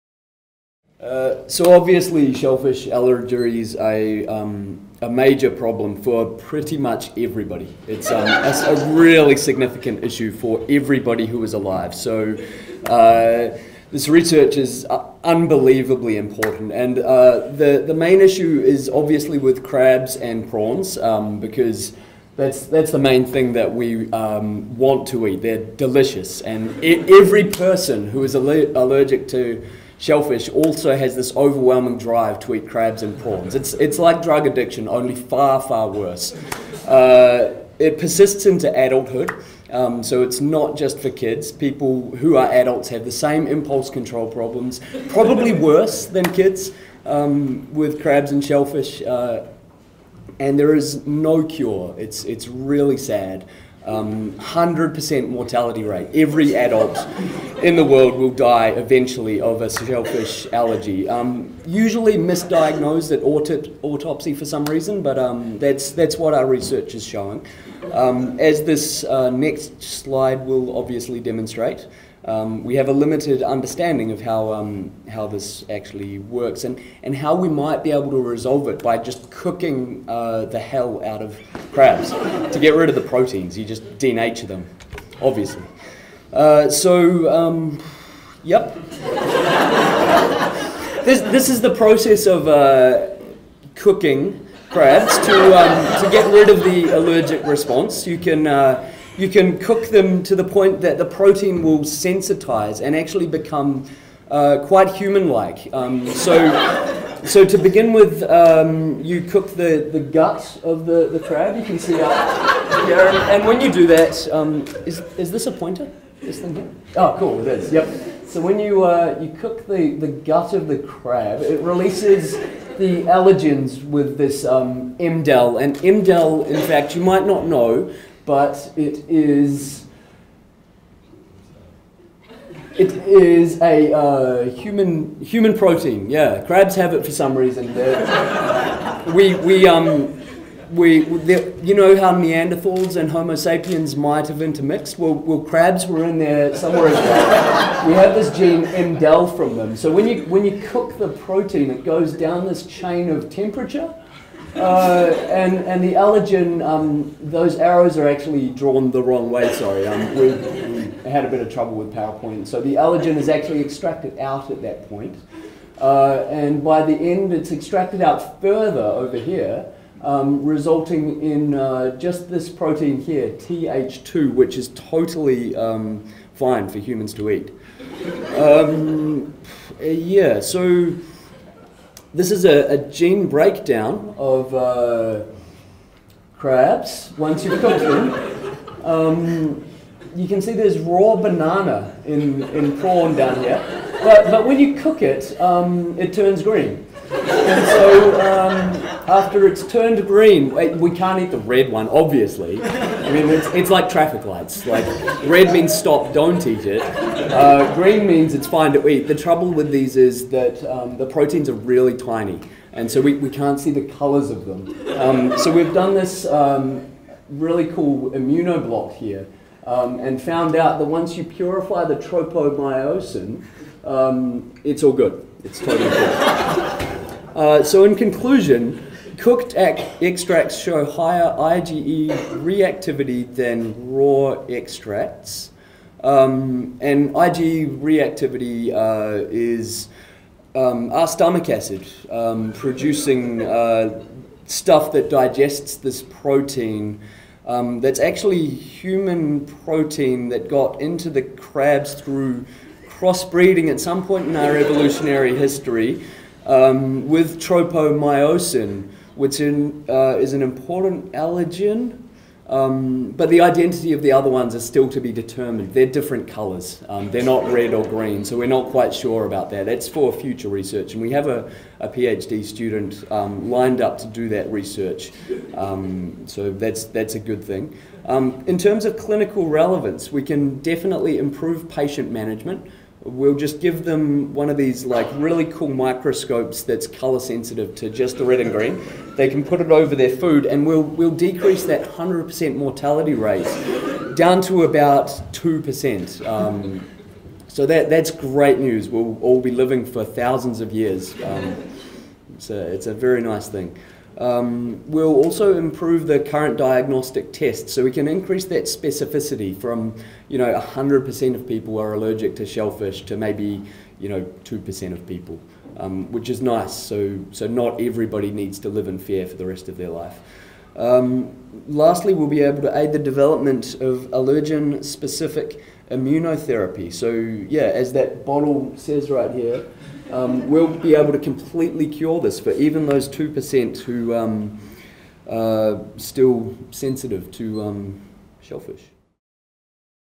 Dr Neil Bailey, MAPrc: "Explain My Graph" challenge
Explain My Graph: In this extempore session, participants are given 3-4 data and summary slides from outside their specialty area to present these slides to the audience.  Dr Neil Bailey (whose real speciality is mindfulness research, gallantly tries to explain the immunology of shellfish allergy at the 2014 CCS Postgraduate symposium.